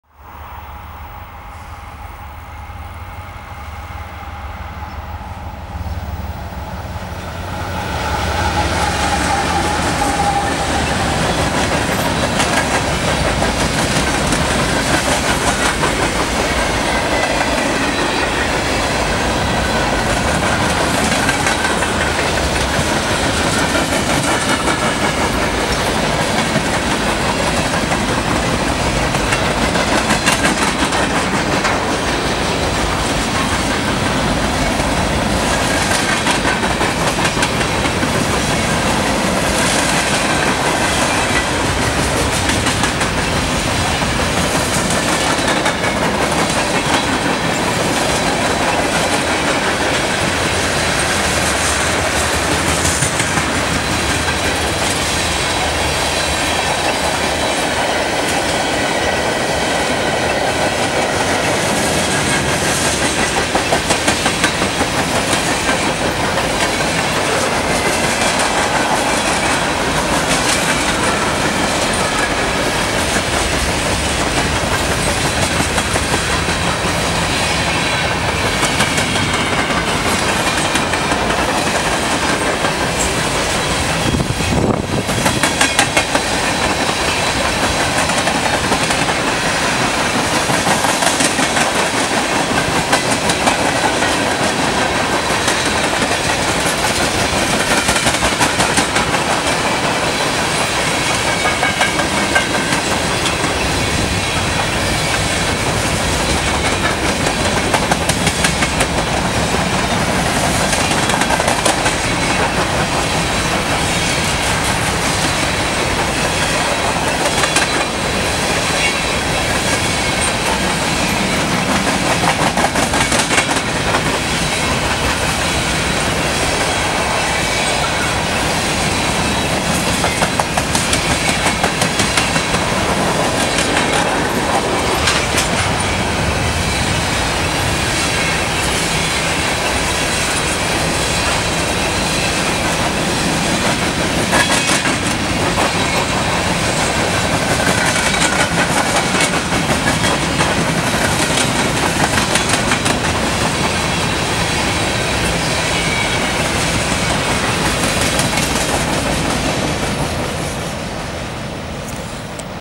Westbound BNSF mixed freight viewed near Gilbert Park in Downers Grove, IL
While trackside taking photos of MoW equipment, a freight train rolled by. I generally video all of a mixed freight because I like to document the freight car mix.